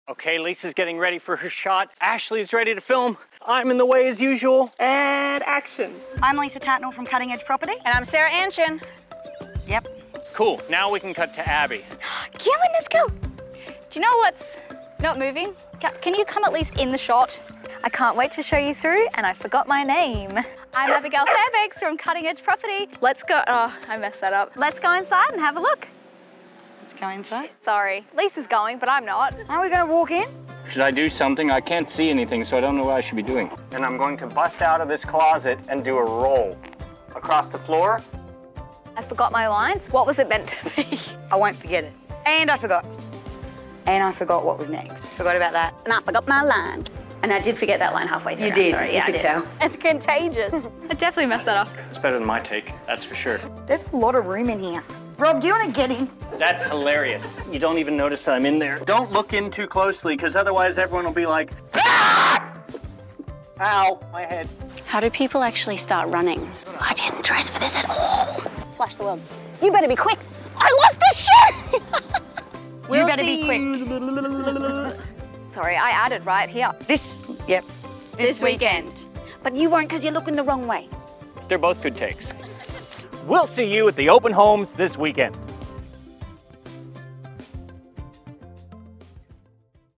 🚫 🙅 These definitely didn't make the final video! 🎥
🍿 It's fun watching us film and if it's your house, You get front row seats! 😄 Here's a look at our last couple of months of bloopers! I'm always goofy.. 😜 But it's super fun to see my serious team 💃 cutting loose when I'm not around.